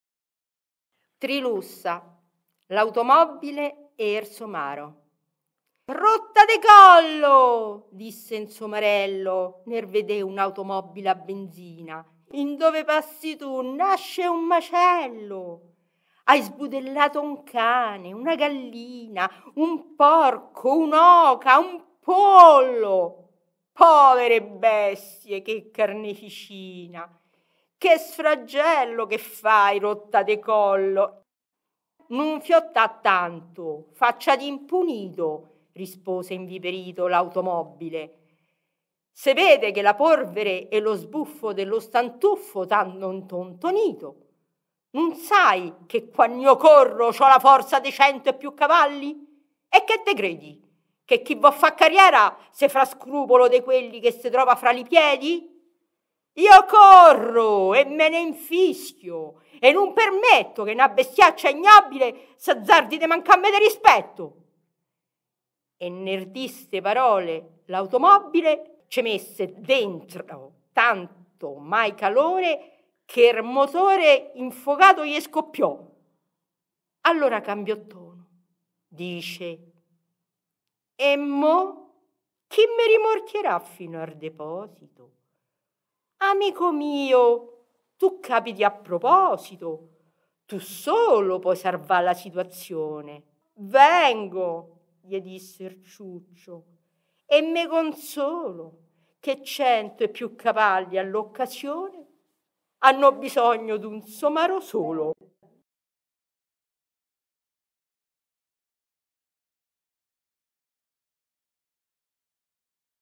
TRILUSSA - L'automobbile e er somaro - Voce: Paola Piali - Videomaker: Mario Michele Orlandi
— Rottadecollo ! — disse un Somarello
ner vede un Automobbile a benzina —
Indove passi tu nasce un macello!
Hai sbudellato un cane, una gallina,
un porco, un'oca, un pollo...
Povere bestie! Che carneficina!
Che sfraggello che fai! Rottadecollo!
— Nun fiottà tanto, faccia d'impunito!
— rispose inviperito l'Automobbile —
Se vede che la porvere e lo sbuffo
de lo stantuffo t'hanno intontonito!
Nun sai che quann'io corro ciò la forza
de cento e più cavalli? E che te credi
che chi vô fa' cariera se fa scupolo
de quelli che se trova fra li piedi?
Io corro e me n'infischio, e nun permetto
che 'na bestiaccia ignobbile
s'azzardi de mancamme de rispetto! —
E ner di' 'ste parole l'Automobbile
ce mésse drento tanto mai calore
che er motore, infocato, je scoppiò.
Allora cambiò tono. Dice: — E mó?
Chi me rimorchierà fino ar deposito?
Amico mio, tu capiti a proposito,
tu solo pôi sarvà la situazzione...
— Vengo, — je disse er Ciuccio — e me consolo
che cento e più cavalli a l'occasione
hanno bisogno d'un Somaro solo!

TRILUSSA - L'automobbile e er somaro
Voce: Paola Piali
Videomaker: Mario Michele Orlandi

Trilussa, pseudonimo anagrammatico di Carlo Alberto Camillo Salustri, è stato un poeta, scrittore e giornalista italiano vissuto tra la fine del XIX e la prima metà del XX secolo. È particolarmente noto per le sue composizioni in dialetto romanesco. Trilussa ha caratterizzato la sua produzione artistica con uno stile satirico capace di evidenziare i difetti e le debolezze dell’animo umano.

Trilussa è nato a Roma il 26 ottobre 1871 da Vincenzo Salustri e Carlotta Poldi. Ha frequentato le scuole municipali San Nicola e il Collegio Poli dei Fratelli delle scuole cristiane, ma ha abbandonato gli studi formali nel 1886 . Trilussa è stato un poeta originale e famoso ancora oggi grazie ai suoi componimenti poetici scritti in dialetto romanesco .

Trilussa è stato un poeta originale e famoso ancora oggi grazie ai suoi componimenti poetici scritti in dialetto romanesco. Ha caratterizzato la sua produzione artistica con uno stile satirico capace di evidenziare i difetti e le debolezze dell’animo umano. Alcune delle sue poesie più famose includono “La tartaruga”, “Nummeri”, “Avarizia” e “La lumachella de la Vanagloria”.

Trilussa è stato anche un giornalista e ha lavorato per diverse testate giornalistiche. Nel 1950 è stato nominato senatore a vita della Repubblica Italiana dal presidente Luigi Einaudi .